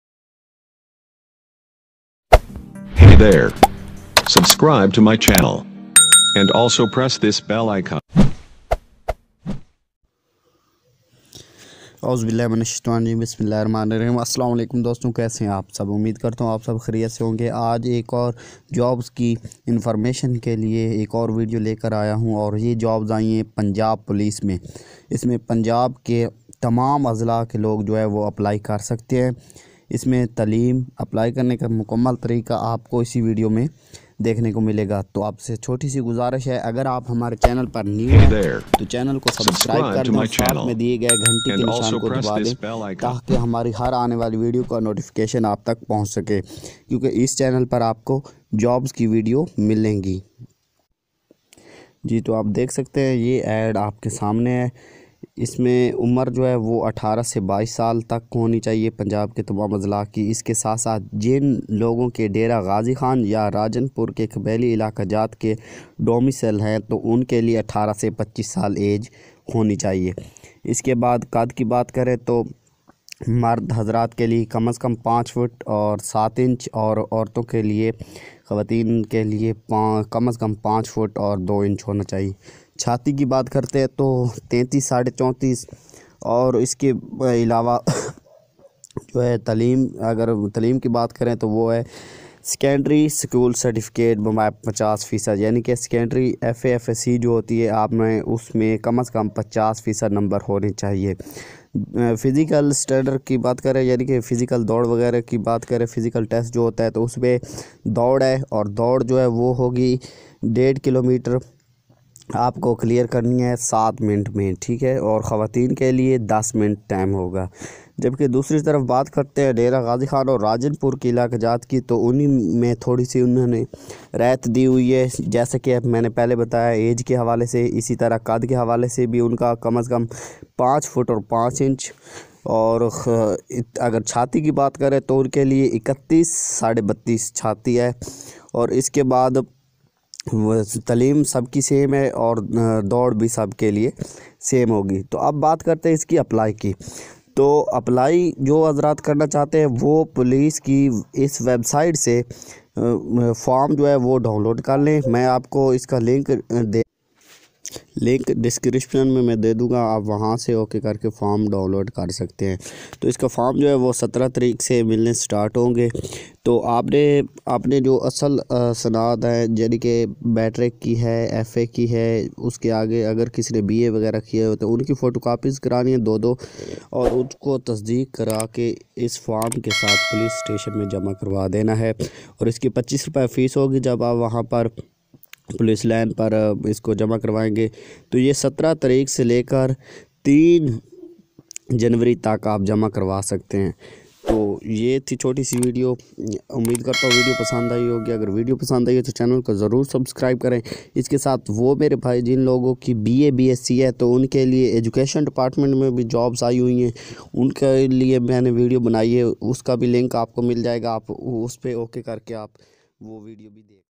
Punjab police jobs |How to apply for police|punjab police application form
Latest jobs in punjab police
Police department jobs pory pakistan say apply kran education physical all details are in this video
How to apply on punjab police jobs